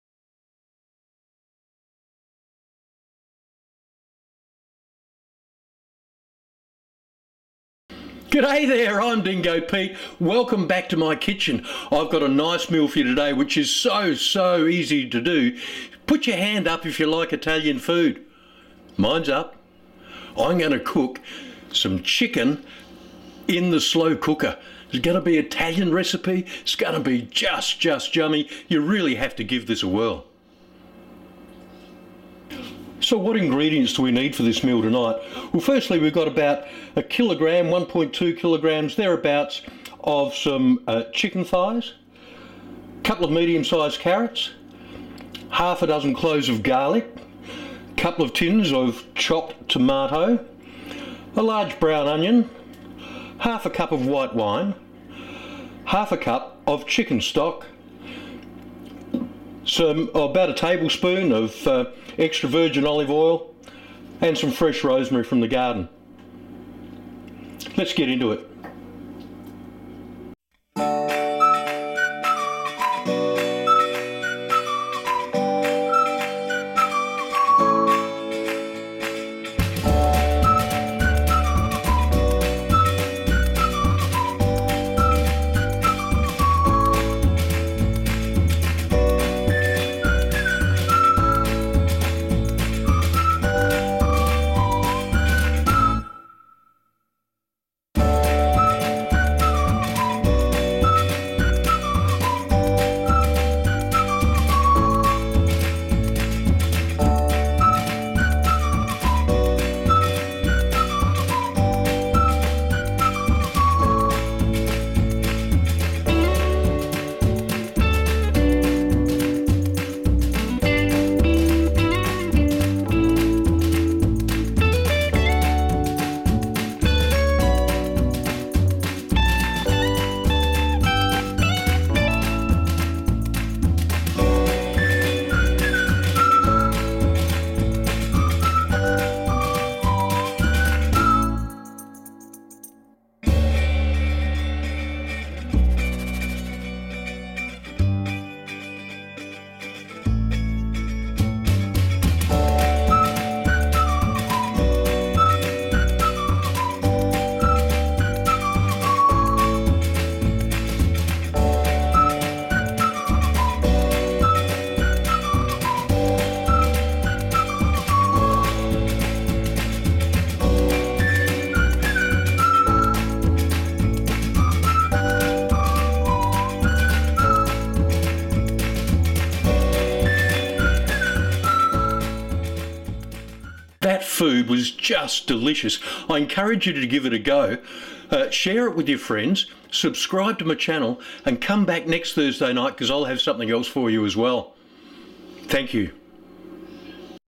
Slow Cooker Italian Chicken
This recipe is incredibly simple to do but the flavours are amazing.  I'm all about simplicity in the kitchen.  Delicious and nutritious food doesn't require a double university degree.  It doesn't require fancy plate work.  All it requires is the right ingredients thrown in at the right time.  You can do this!  It's so easy.